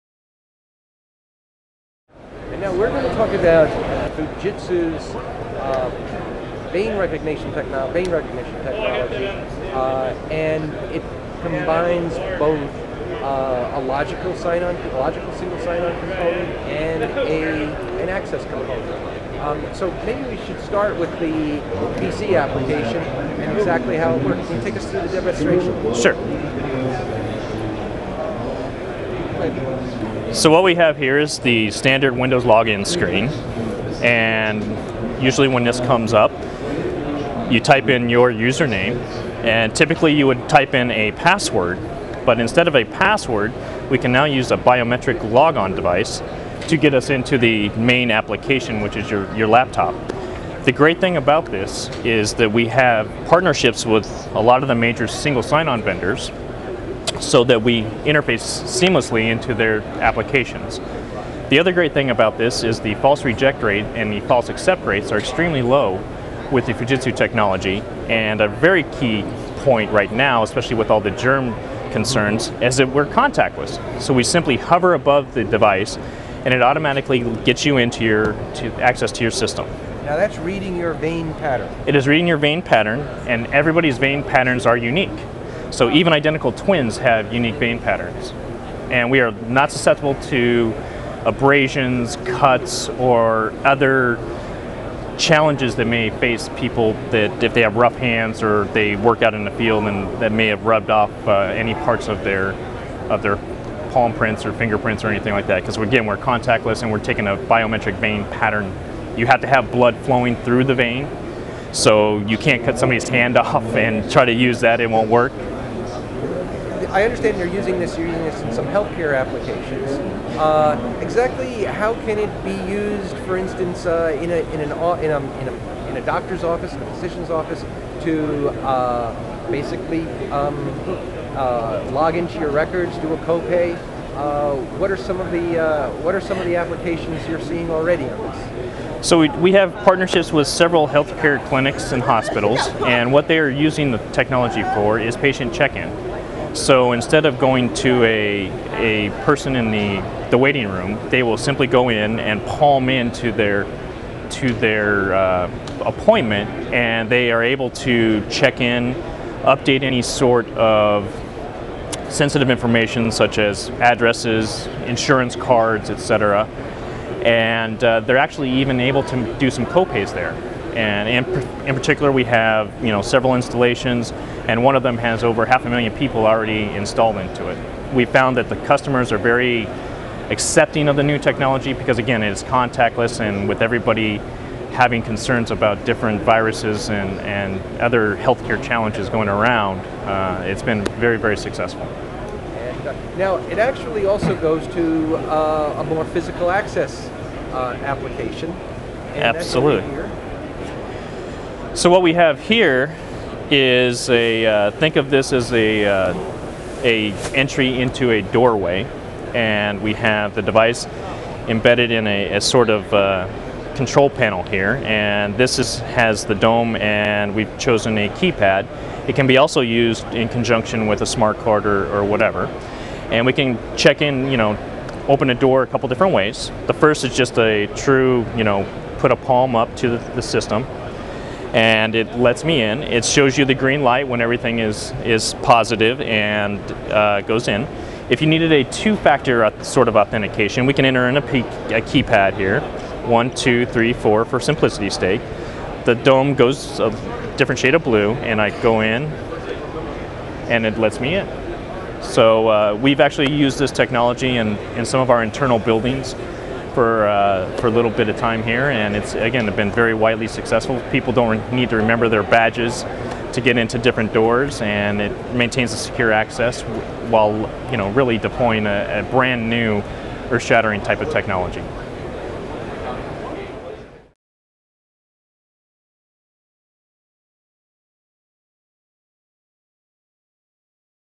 Fujitsu Vein Recognition Technology demo
Jim Preasmyer of Fujitsu Computer Products of America demonstrates the company's Palmsecure vein recognition technology for single sign-on security applications. Taped at 2009 ASIS International exhibits Sept, 23, 2009.